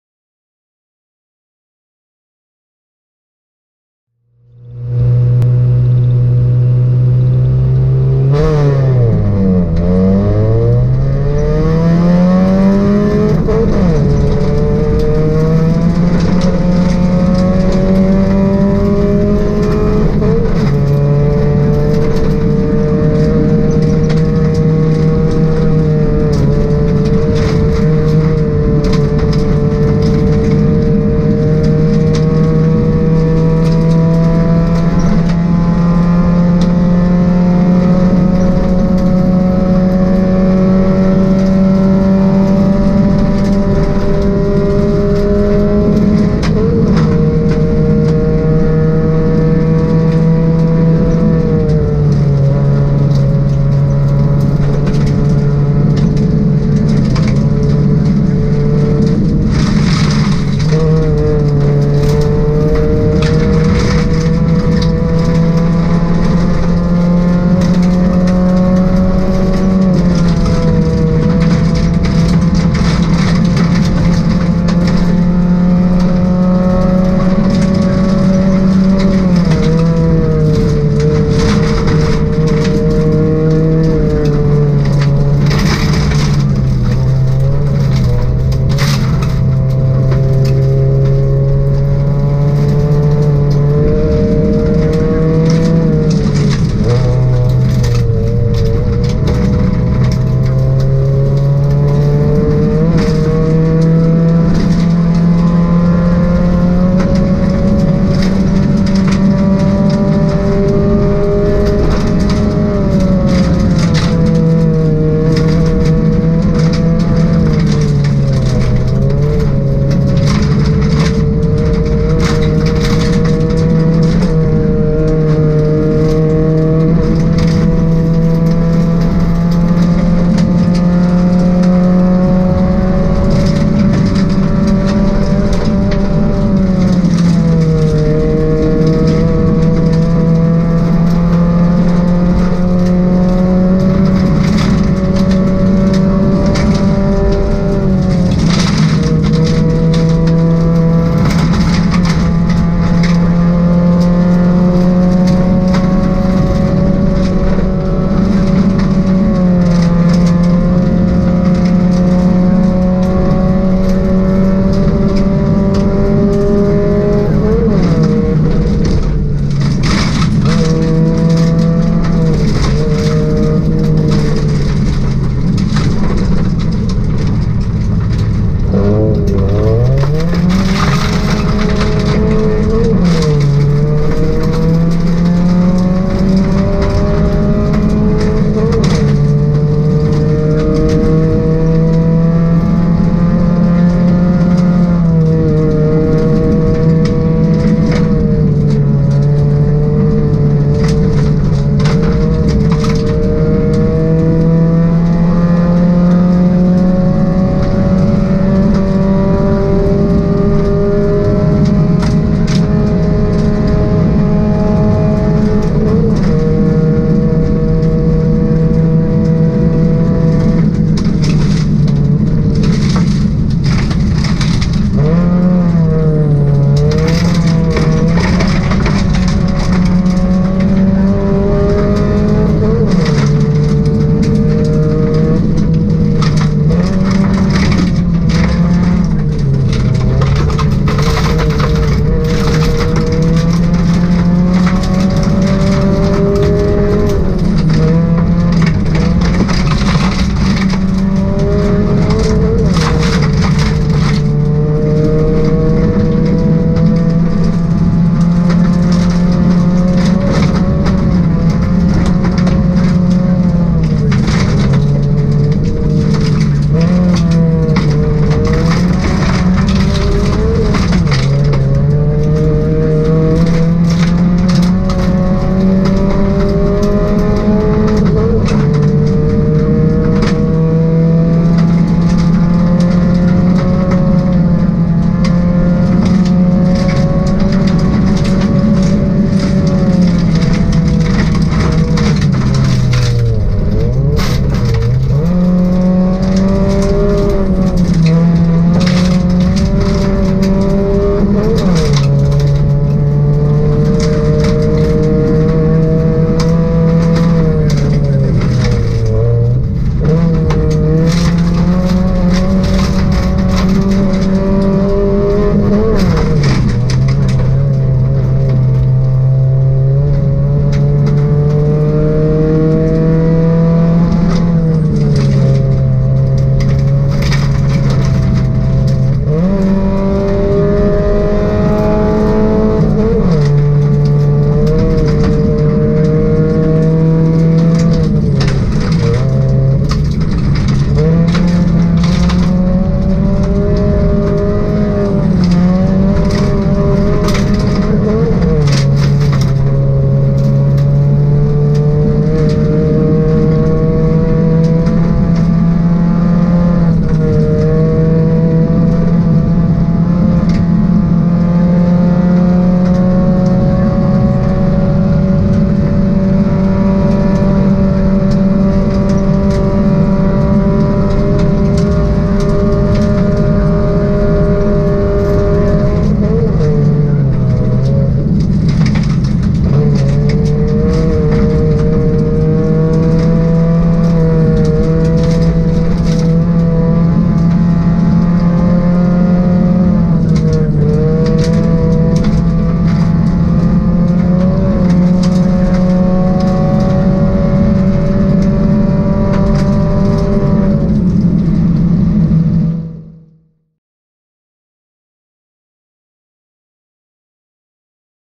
Rengas Turku Ralli 2022 Ek2 #68
Driver: Esko Kytömäki (Age 15)
Co-driver: Niilo Ahonen (Age 19)
Car: Ford Focus V1600